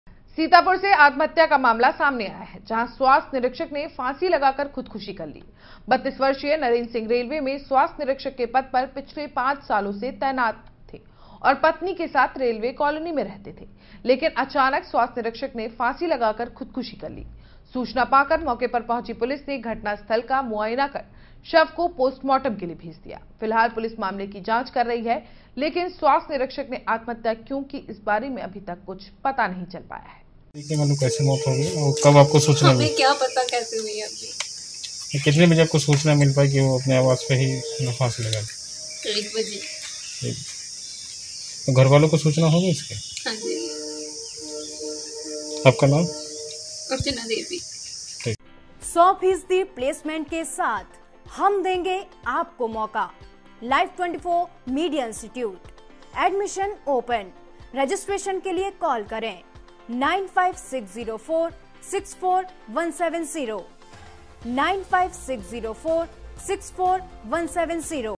सीतापुर में स्वास्थ्य निरीक्षक ने फांसी लगाकर की आत्महत्या
सीतापुर से आत्महत्या का मामला सामने आया है...जहां स्वास्थ्य निरीक्षक ने फांसी लगाकर खुदकुशी कर ली...32 वर्षीय नरेन्द्र सिंह रेलवे में स्वास्थ्य निरीक्षक के पद पर पीछले पांच सालों तैनात से और पत्नी के साथ रेलवे कॉलोनी रहते थे... लेकिन अचनाक स्वास्थ्य निरीक्षक ने फांसी लगाकर खुदकुशी कर ली...सूचना पाकर मौके पर पहुंची पुलिस ने घटना स्थल का मुआयन कर शव को पोस्टमार्टम के लिए भेज दिया... फिलहाल पुलिस मामले की जांच कर रही है... लेकिन स्वास्थ्य निरीक्षक ने आत्महत्या क्यों कि इस बारे अभी कुछ भी पता नहीं चल पाया है...